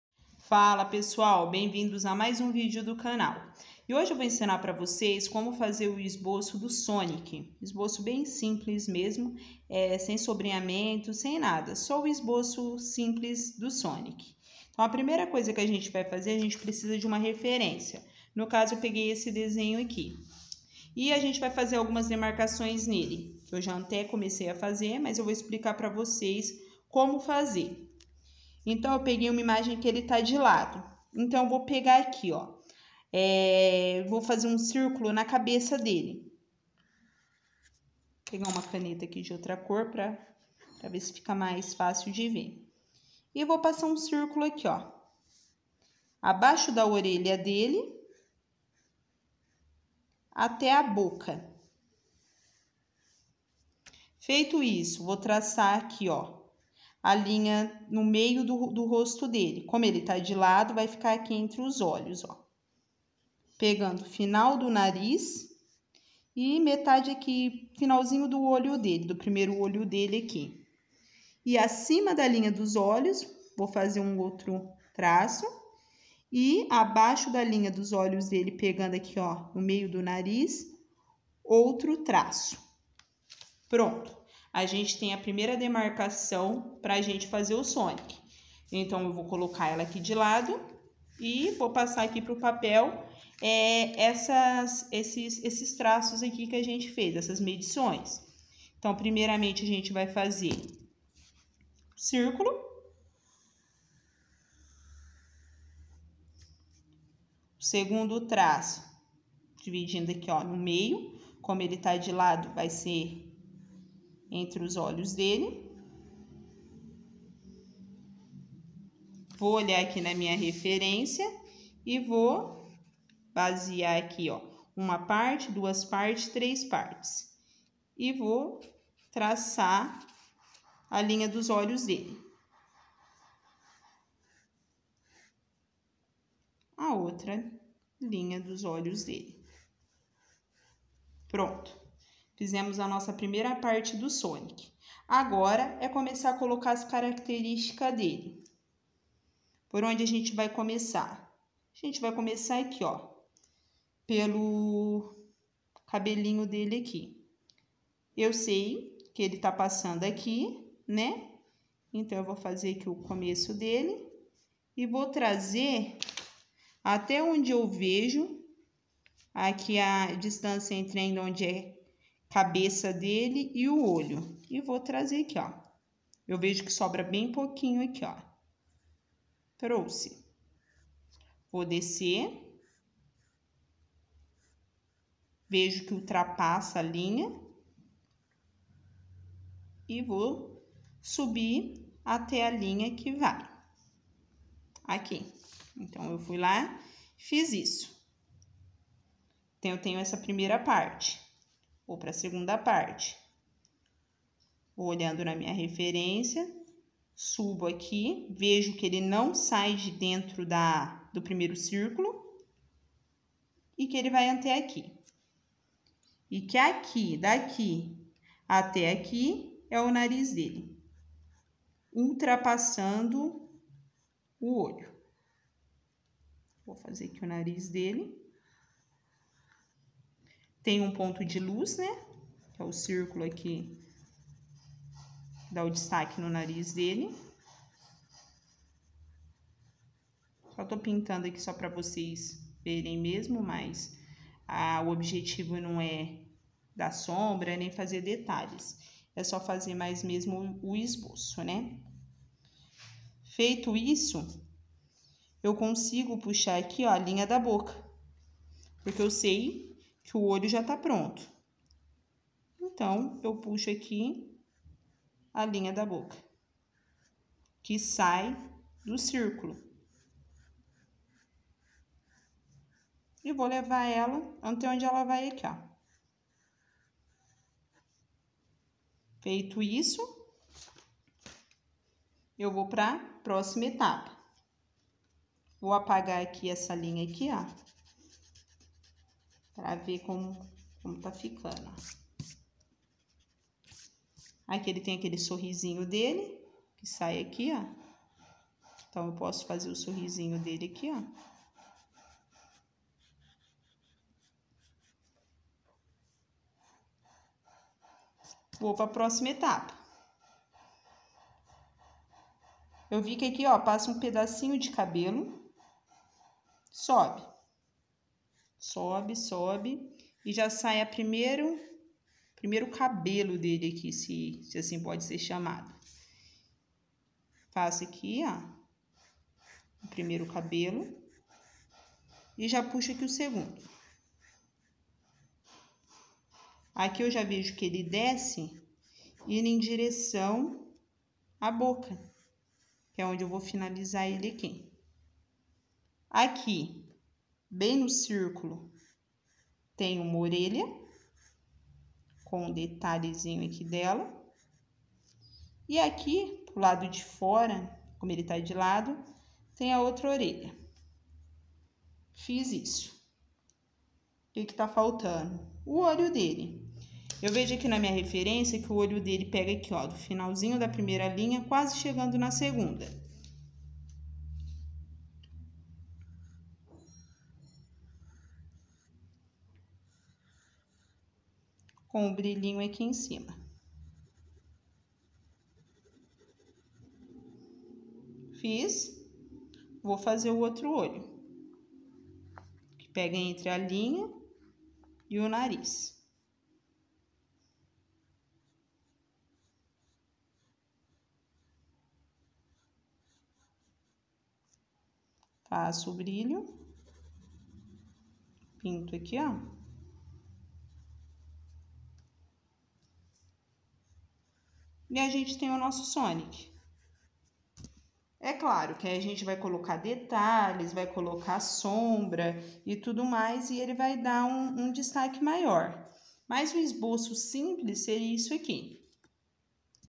Como desenhar o Sonic (esboço simples)
Passo a passo de como desenhar o sonic, desenho simples e facil, para quem esta aprendendo a desenhar
Como Desenhar o Sonic: Passo a Passo para Iniciantes!

Olá, pessoal! Neste vídeo, eu vou mostrar como desenhar o Sonic, o ouriço mais rápido e icônico dos jogos! 🌟🎨

Se você sempre quis aprender a desenhar o Sonic, está no lugar certo. Vou guiar você por cada etapa do processo, desde os esboços iniciais até os detalhes finais. Então, pegue seu papel, lápis e borracha, e vamos começar essa aventura artística juntos!

Deixe seu comentário dizendo o que achou do desenho e qual personagem você quer ver no próximo tutorial. ✏️✨